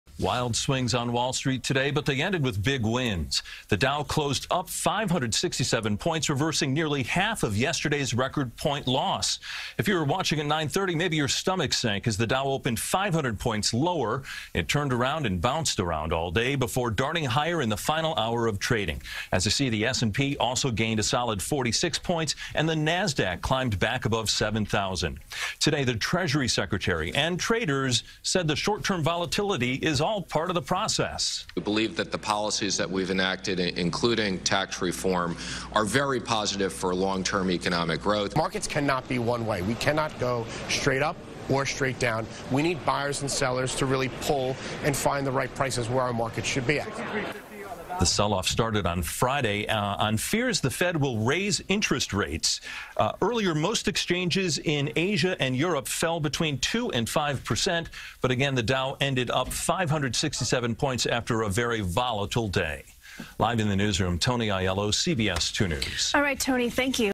Wild Swing On Wall Street
It was another dramatic day on Wall Street as stocks nose-dived and then soared. CBS2's Tony Aiello has the latest.